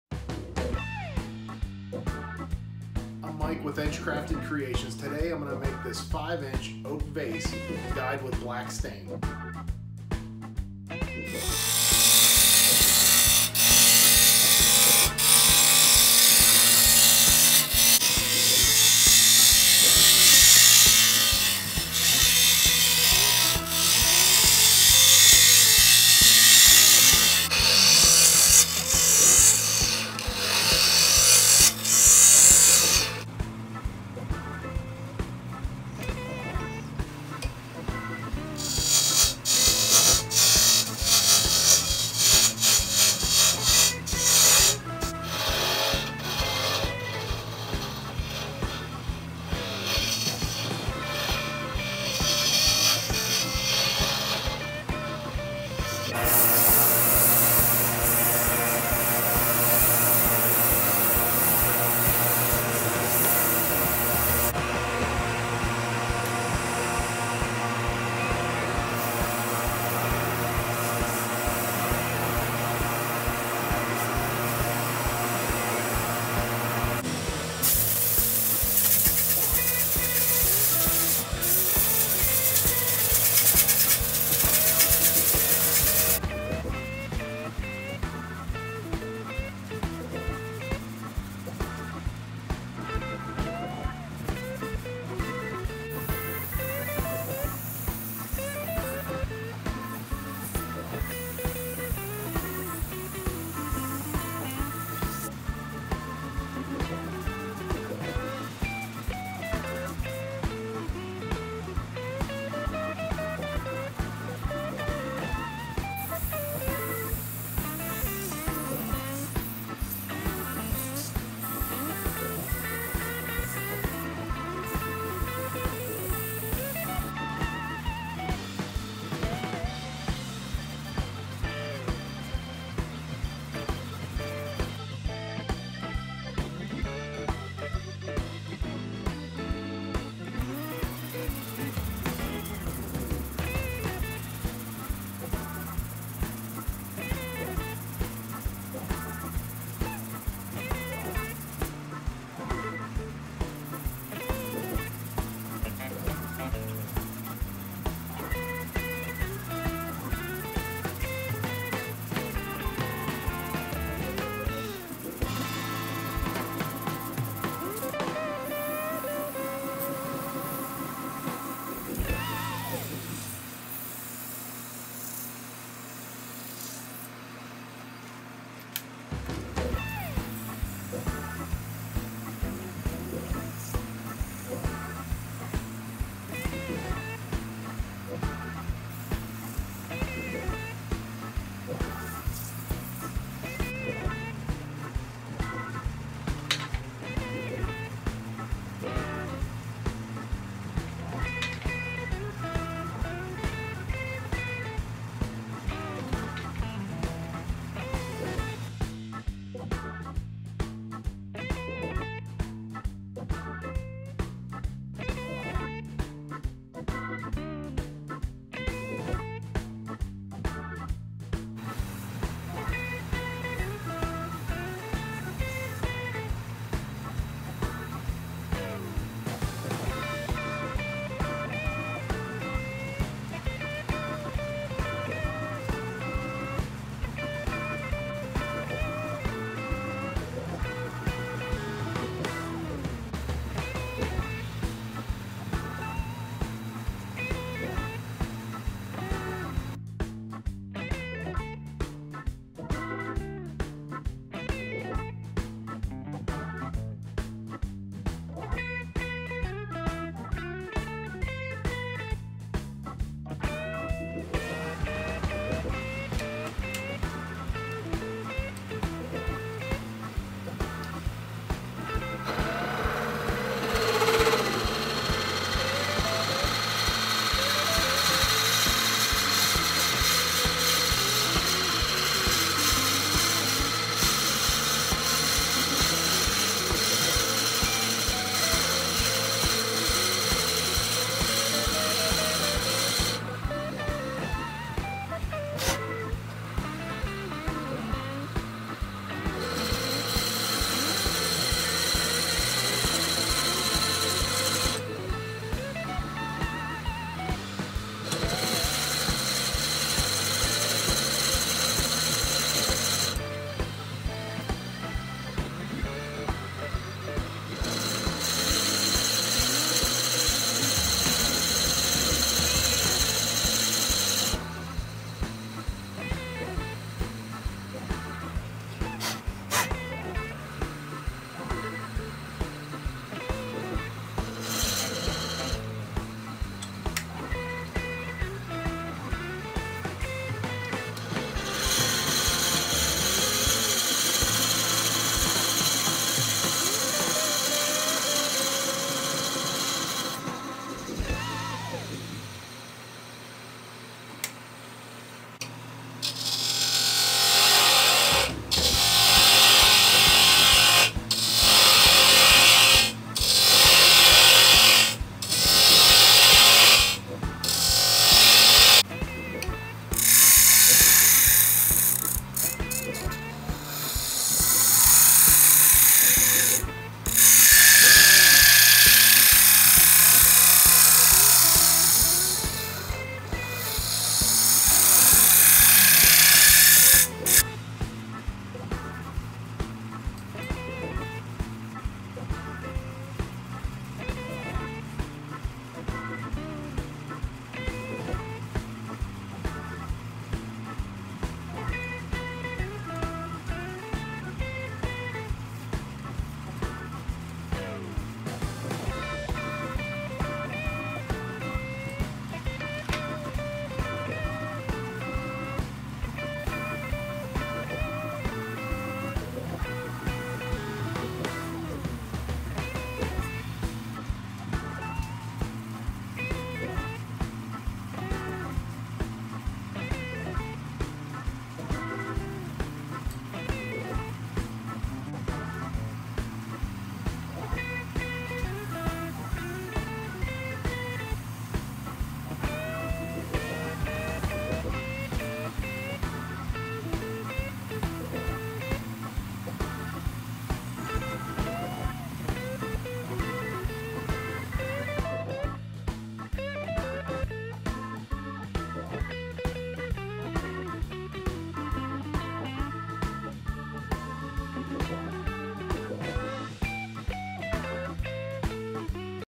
Dyed Oak Vase
Follow along as a beautiful piece of oak is turned into a vase with the grain highlighted by black stain.